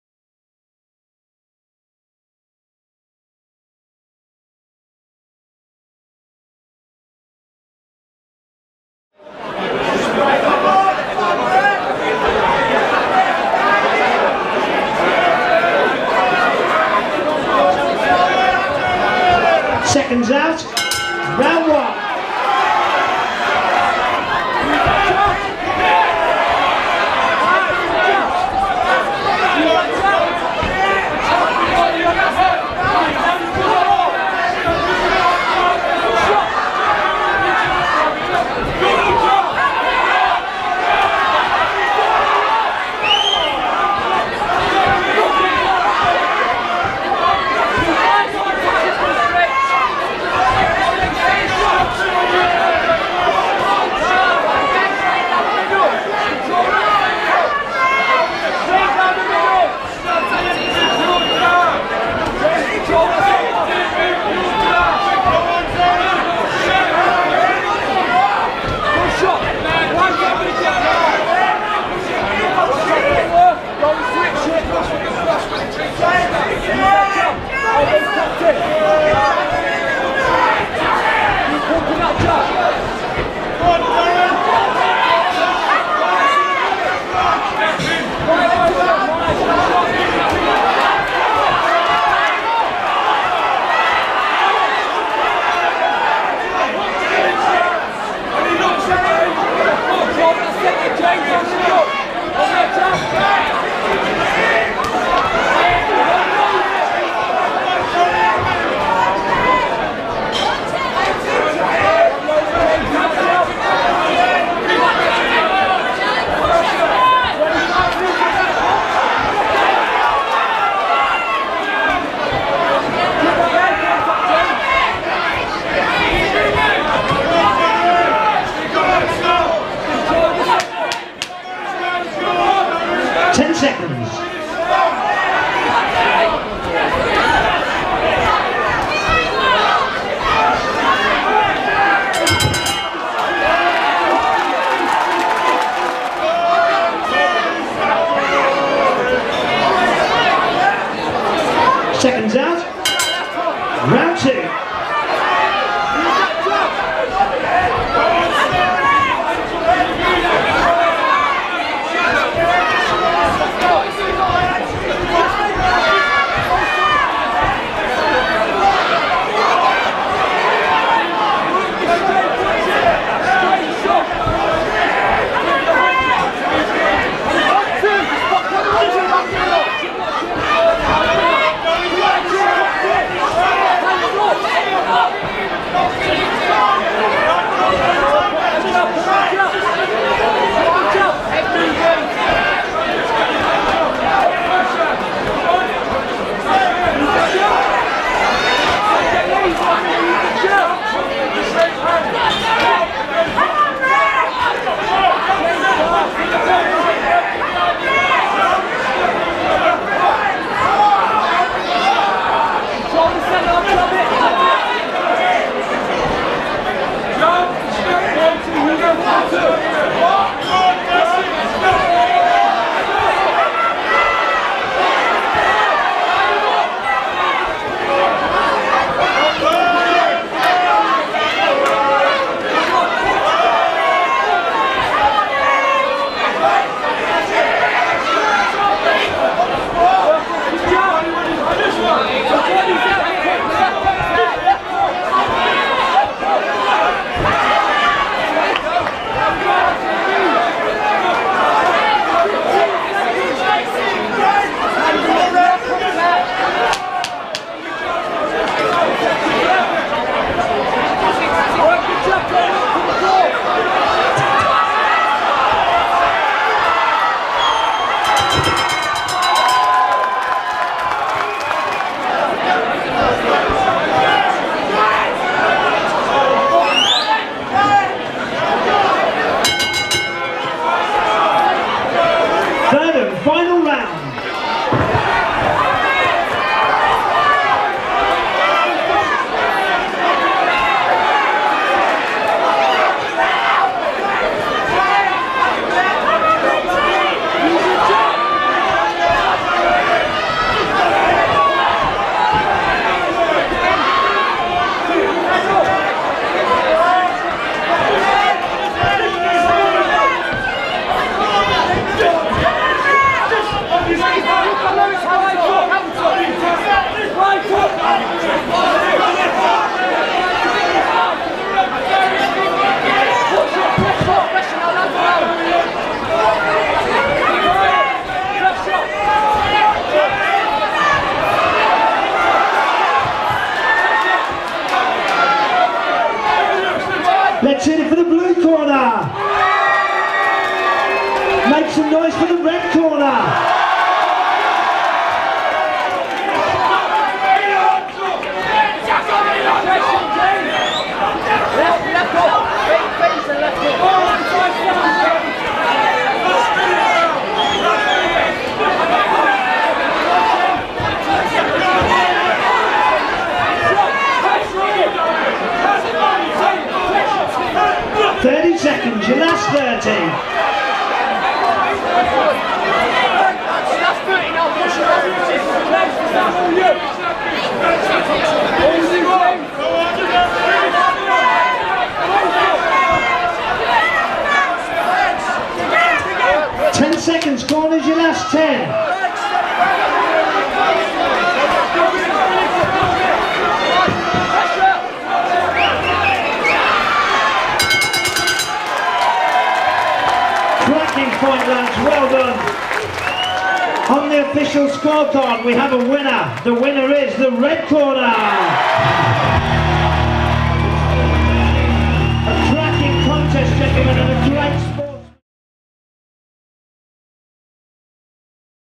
Ultra White Collar Boxing | Stafford | Darius Sanhori VS James Grunda
Stafford Ultra White Collar Boxing 31st March 2017

8 Weeks FREE training / Get in the best shape of your life/ Raise money for Cancer Research UK